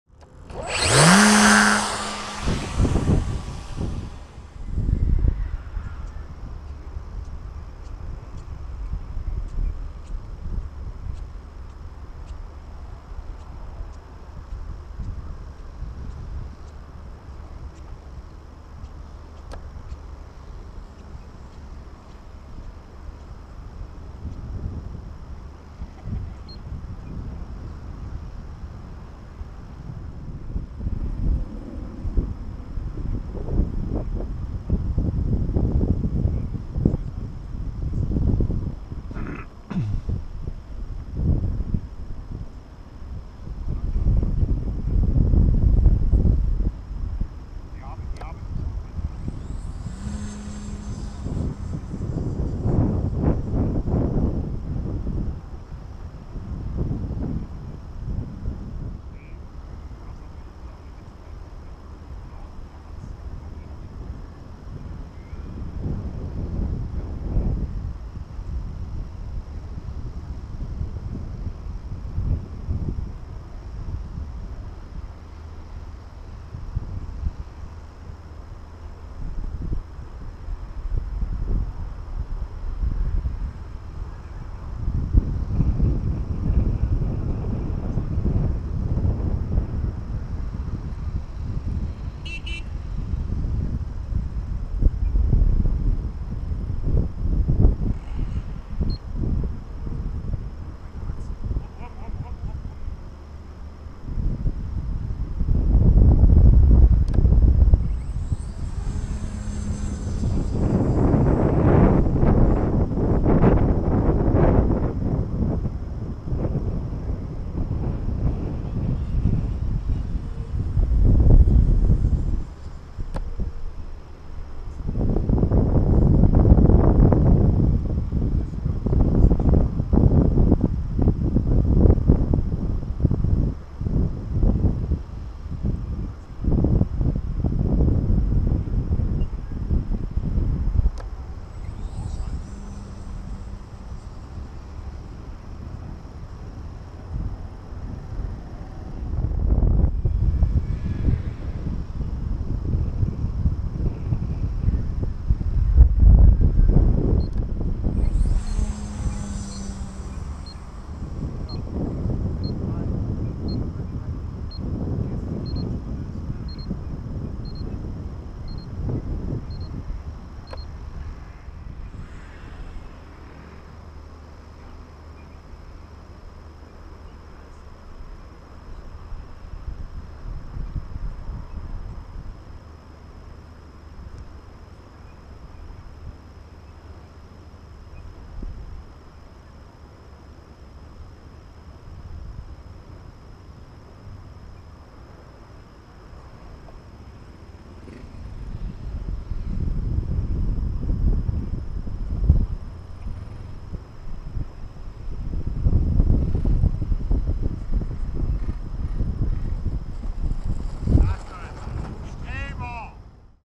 Staufenbiel Vitesse V2 mit 4s
Vitesse V2 von Staufenbiel. Antrieb mit 4s 3000 mAh.
Ein bisschen Spaß haben, bei etwas kräftigerem Wind. Für die Vitesse aber kein Problem.